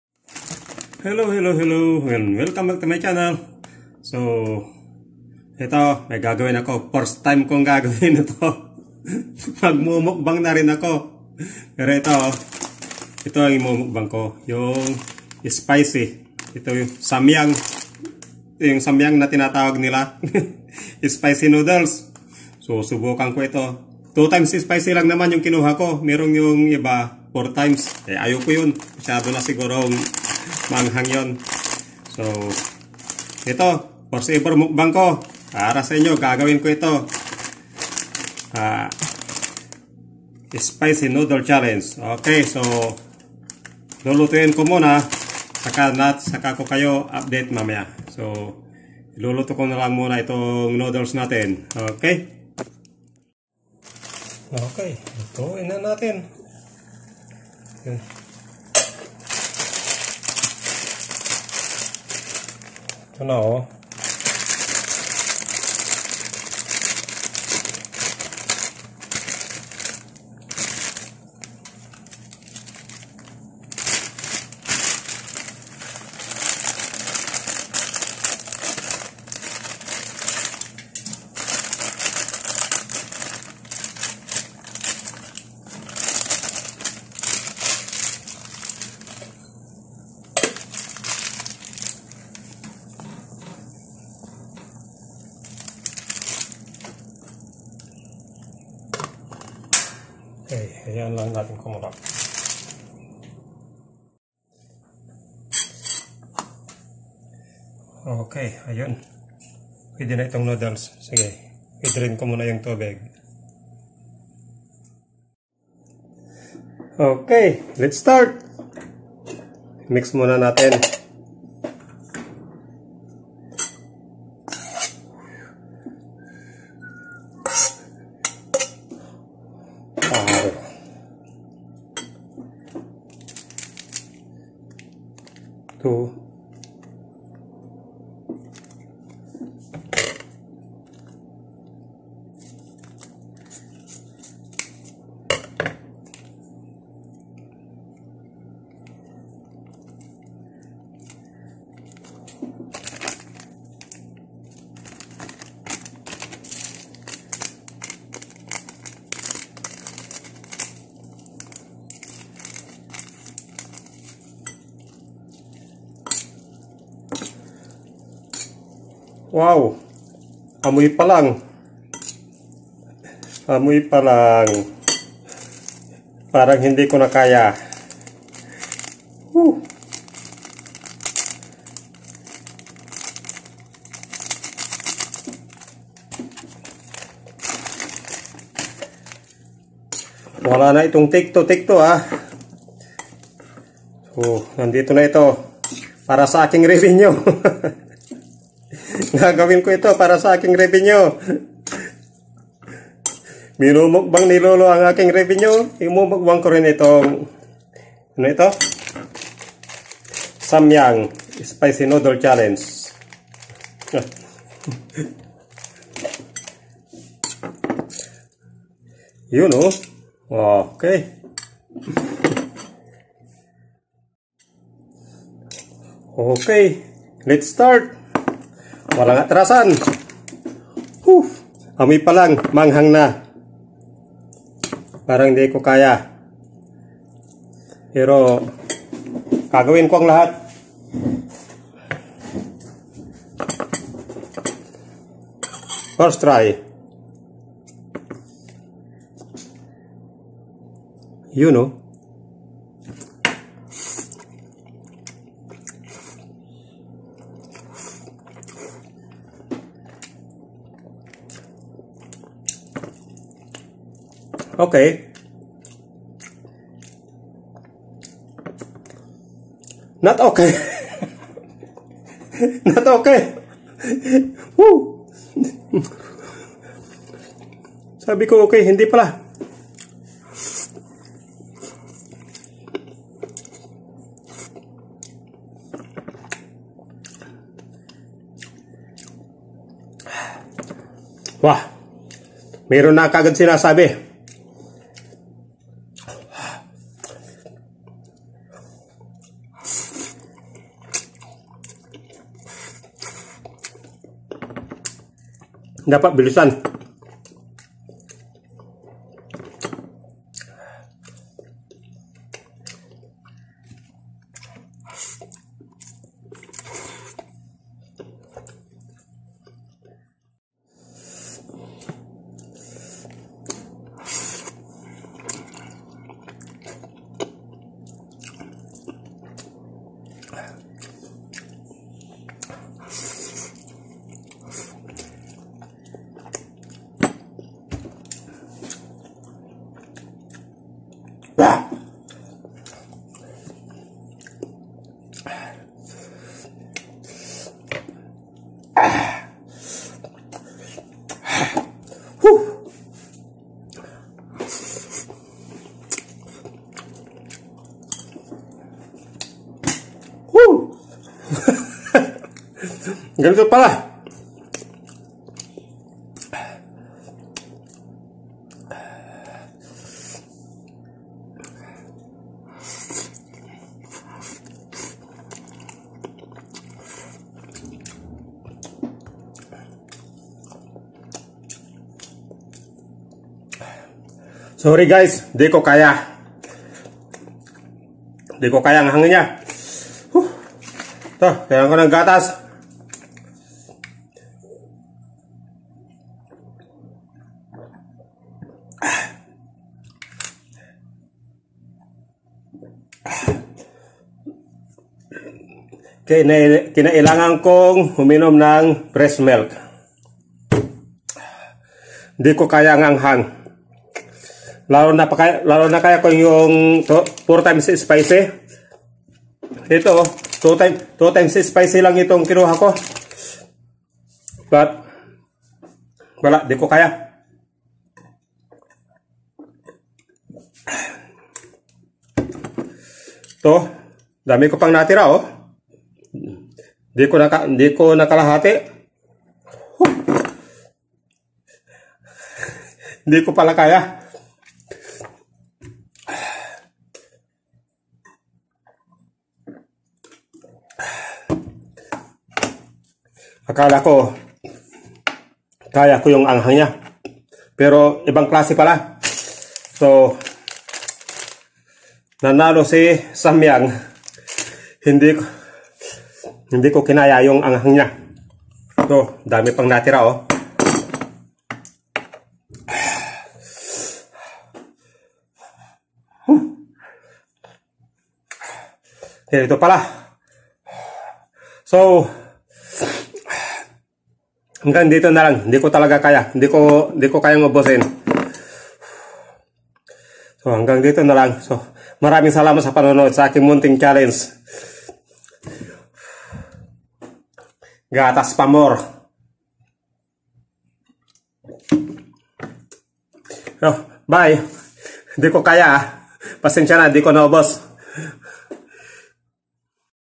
man vs food, spicy noodle challenge
First ever mukbang challenge, eating samyang spicy noodles.